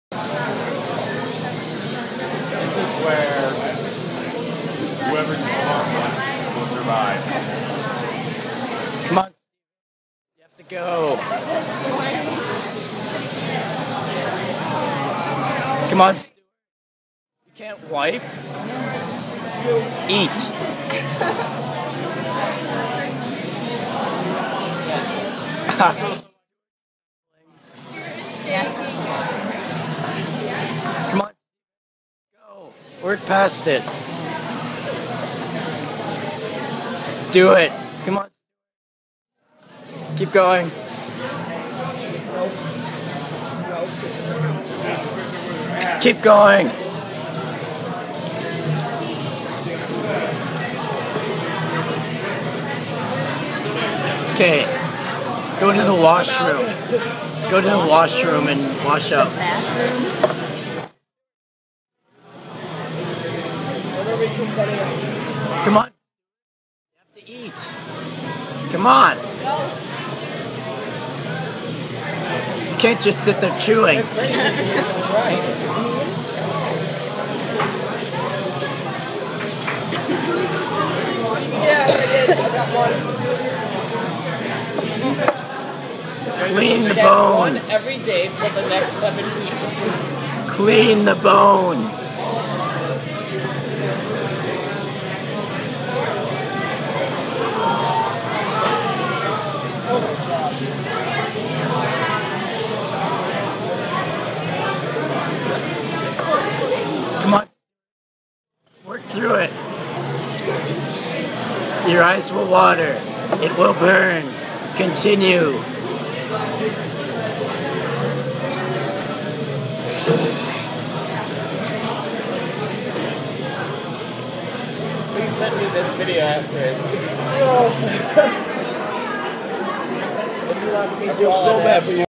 After Game Marching Band Stuff - Bdubs Blazin Challenge
*NOTE: StuCan had 1 extra wing more before the camera started recording

Just me and a couple of friends at Bdubs. I hope you enjoy it. Credit for the recorder (who wasn't me) and a big Thank you to him for letting me use it on my channel.

Additional information not in the video,
1) They both ordered 20 wings
2) They both got glasses of milk for free afterwards